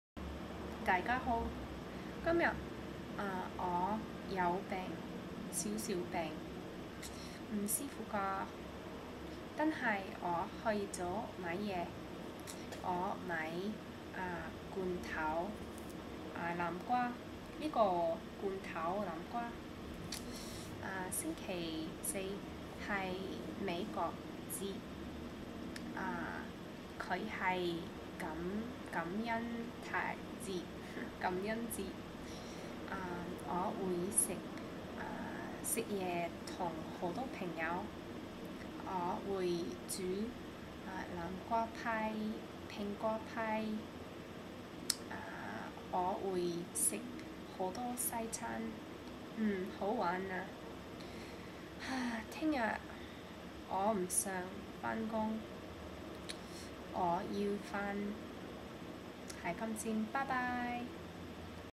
Laura/Cantonese/November13 MLR
Just a simple video talking a little about my day and Thanksgiving (which is coming up).